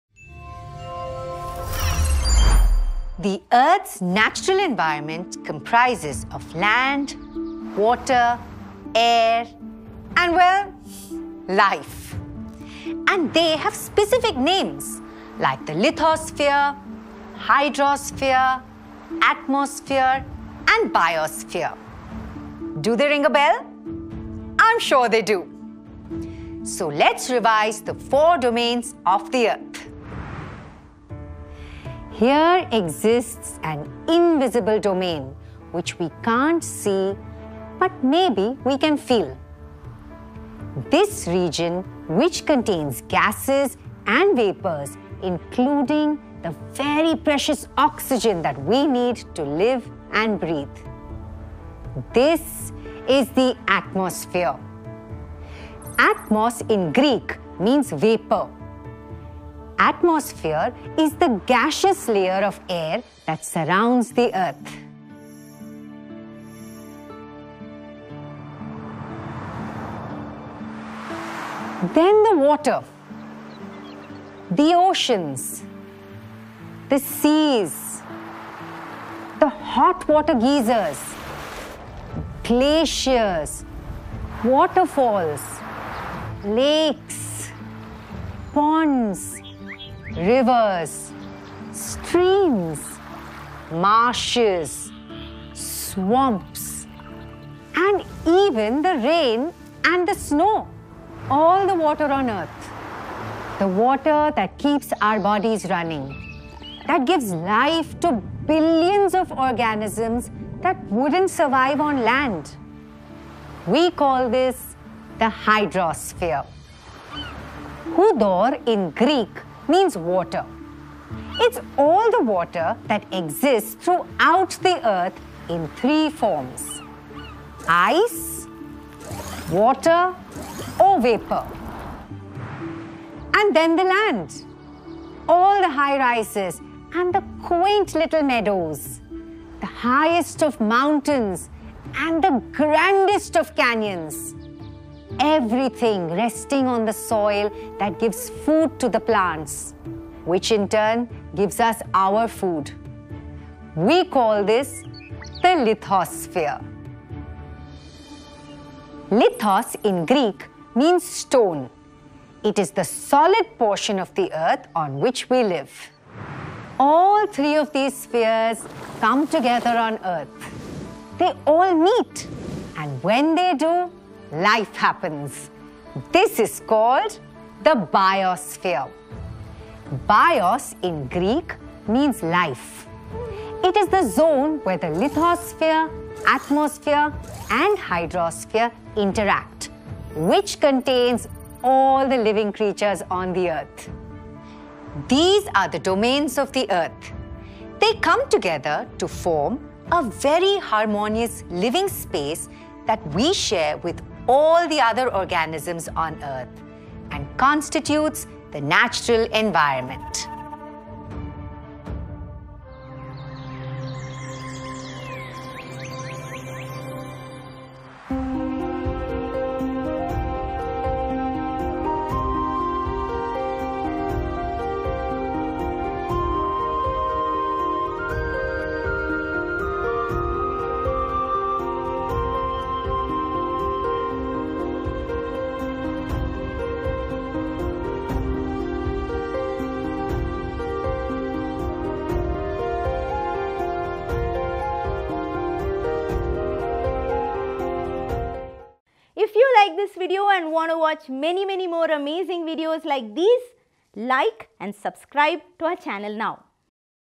The Environment | Class 7 - Geography | Learn with BYJU'S
Look around. We live on a planet which has so many things. Some of the prominent ones include Land, Water, Air and different types of life. All of these comprise our environment. These prominent things are also called by different names. What are those names? Not just these, we are also surrounded by different forms of life. What about them? To find out more, watch this video.

💸 Cash Rewards
🏆 Be an All-India Rank